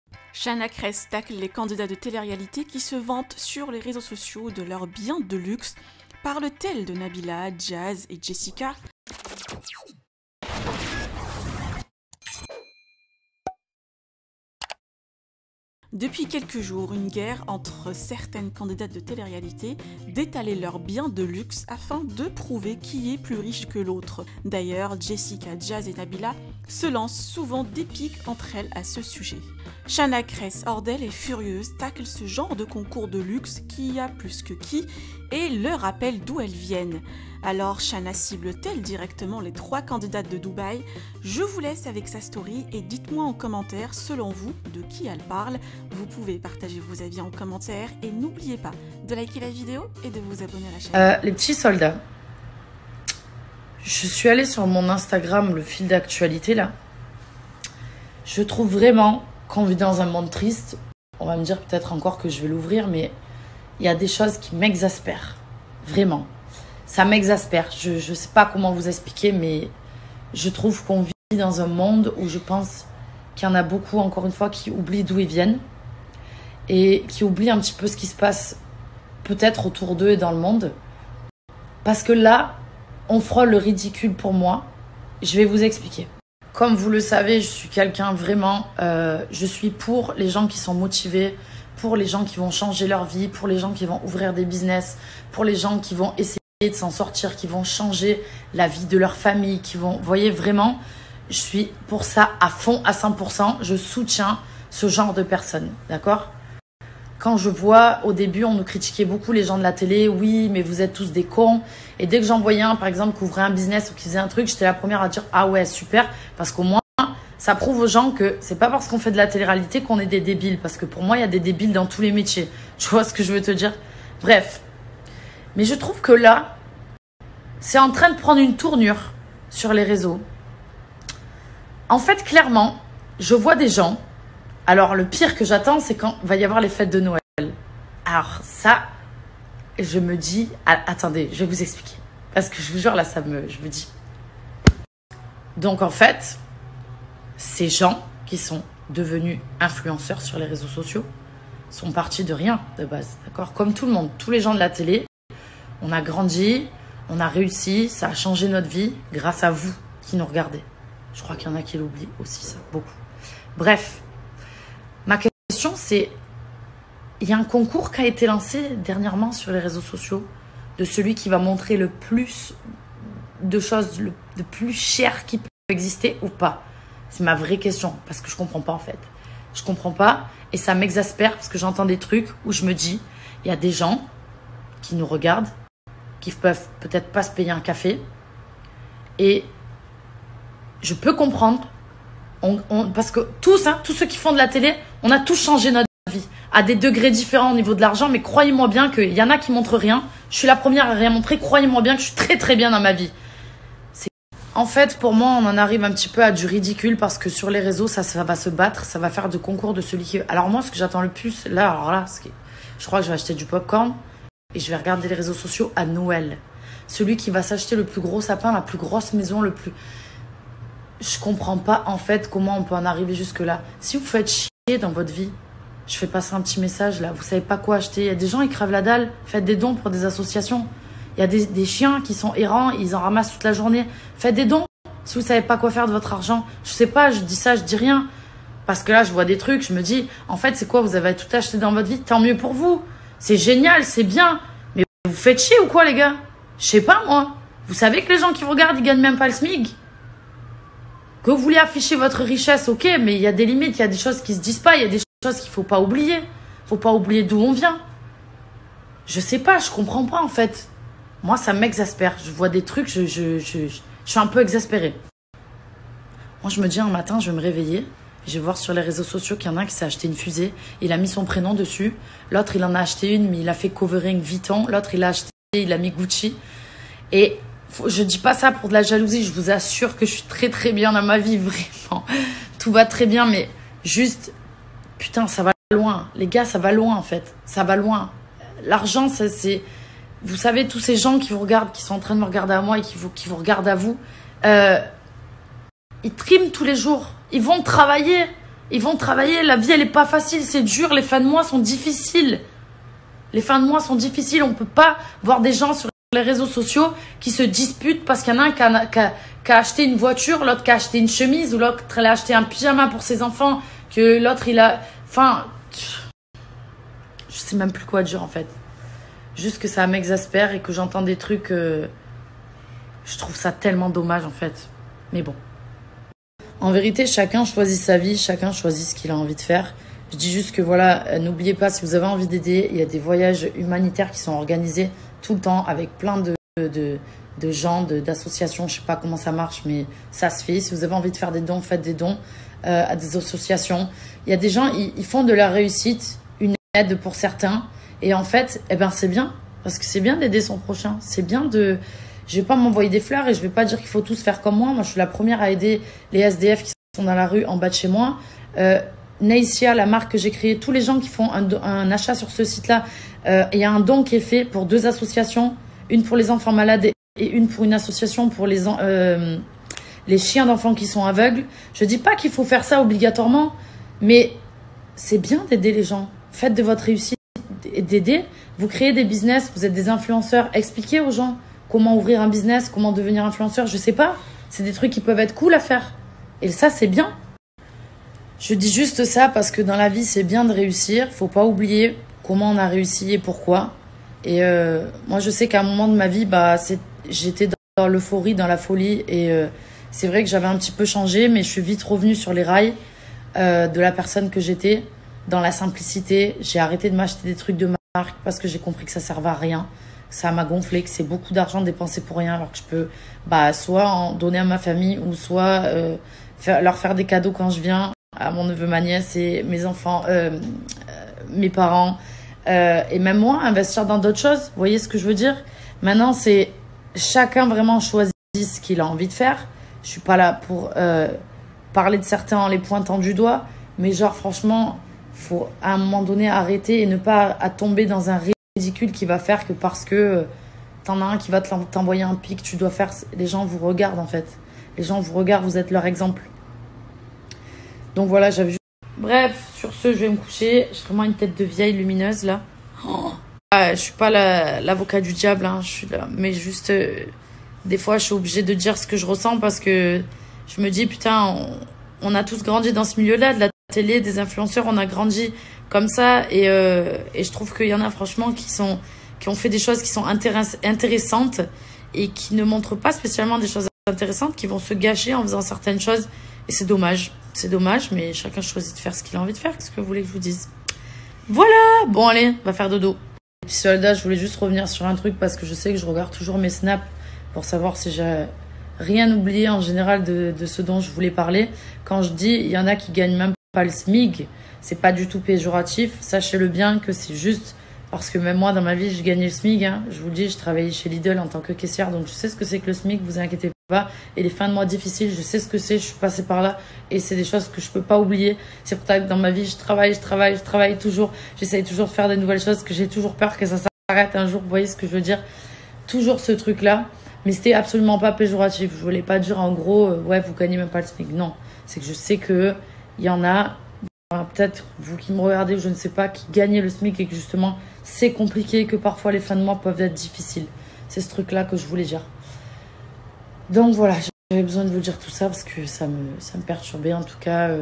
SHANNA KRESS TACLE JESSICA, NABILLA ET JAZZ SUR LEUR AFFICHAGE DE BIENS DE LUXE?!
Shanna Kress sans les noms, pointe du doigt ce concours de luxe et leur rappelle d'où elles viennent!
Alors cible t-elle les 3 candidates de Dubai?